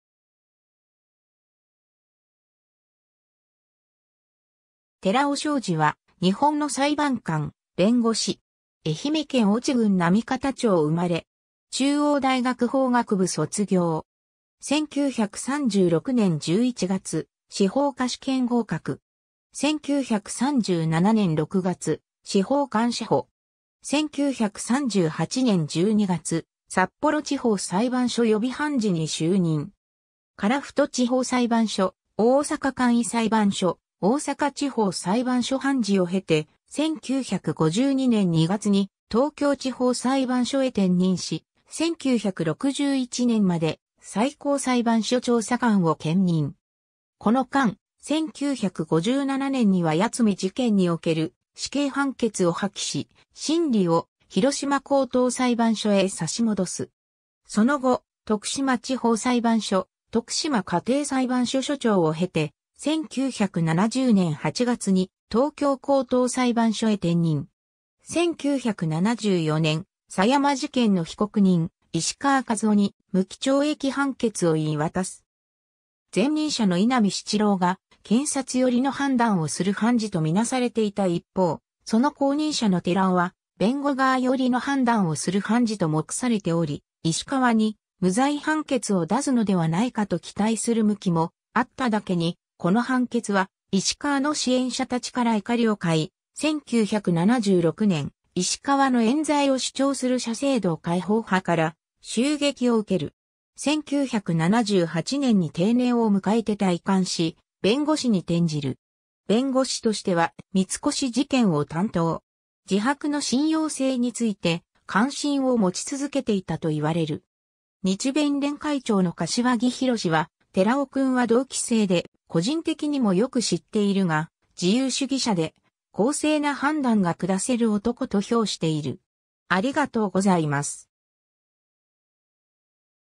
寺尾正二
寺尾 正二（てらお しょうじ、1913年7月13日 - ）は日本の裁判官、弁護士。
 愛媛県越智郡波方町（現・今治市）生まれ。
中央大学法学部卒業。
1936年11月、司法科試験合格。
1937年6月、司法官試補（東京）。
1938年12月、札幌地方裁判所予備判事に就任。
樺太地方裁判所、大阪簡易裁判所、大阪地方裁判所判事を経て、1952年2月に東京地方裁判所へ転任し、1961年まで最高裁判所調査官を兼任。
この間、1957年には八海事件における死刑判決を破棄し、審理を広島高等裁判所へ差し戻す。
 その後、徳島地方裁判所・徳島家庭裁判所所長を経て、1970年8月に東京高等裁判所へ転任。
1974年、狭山事件の被告人 石川一雄に無期懲役判決を言い渡す。
前任者の井波七郎が検察寄りの判断をする判事とみなされていた一方、その後任者の寺尾は弁護側寄りの判断をする判事と目されており、石川に無罪判決を出すのではないかと期待する向きもあっただけにこの判決は石川の支援者たちから怒りを買い、1976年、石川の冤罪を主張する社青同解放派（の学生組織である反帝学評）から襲撃を受ける（東京高裁判事襲撃事件）。
1978年に定年を迎えて退官し、弁護士に転じる。
弁護士としては三越事件を担当。
自白の信用性について関心を持ち続けていたといわれる。
 日弁連会長の柏木博は「寺尾君は同期生で個人的にもよく知っているが、自由主義者で、公正な判断が下せる男」と評している。